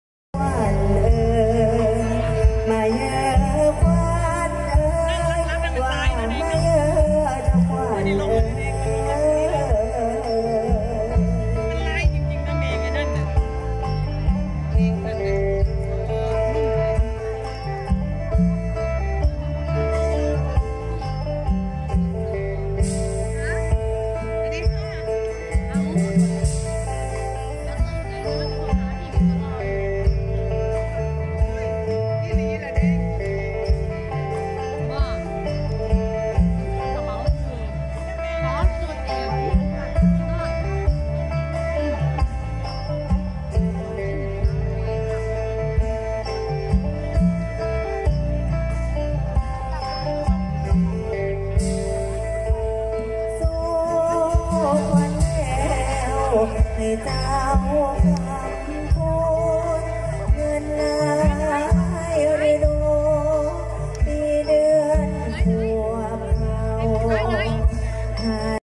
29ตุลาคม2567การแสดงสมโภชกฐินพระราชทาน ณ ค่ายลูกเสือจังหวัดศรีสะเกษ(ห้วยคล้า)
การแสดงสมโภชกฐินพระราชทาน ณ ค่ายลูกเสือจังหวัดศรีสะเกษ(ห้วยคล้า)